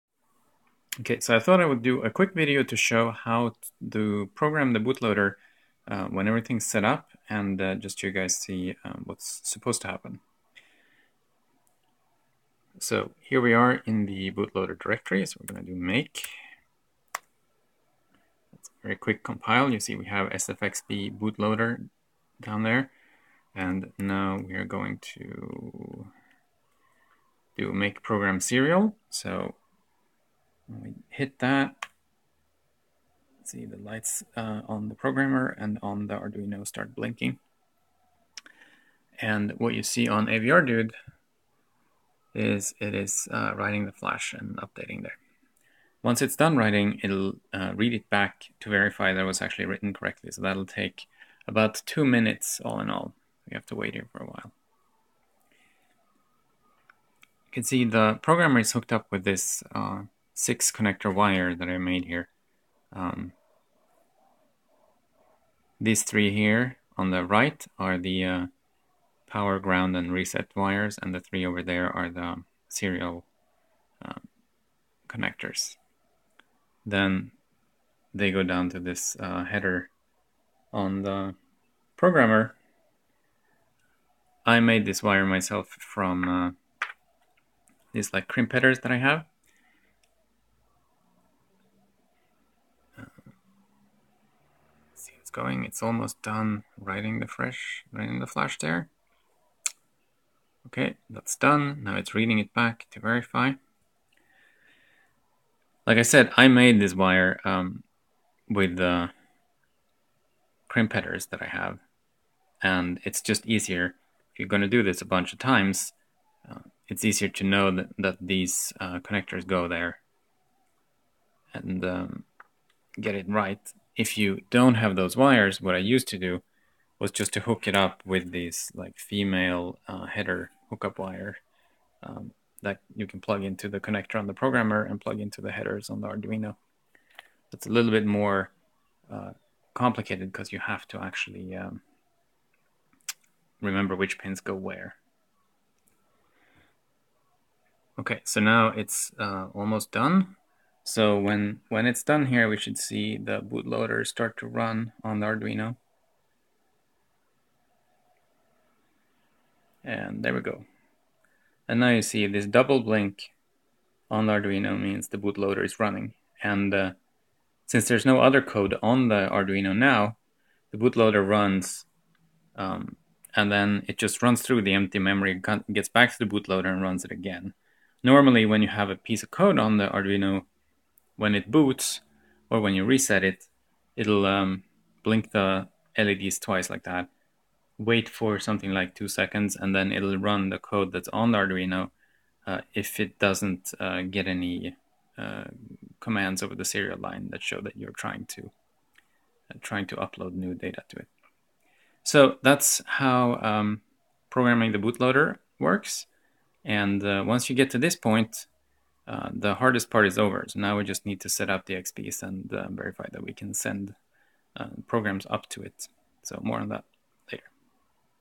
Writing the sfxb bootloader to an Arduino
This video shows how writing the sfxb bootloader to an Arduino Pro Mini works. This is a companion video to my blog post about wireless bootloading.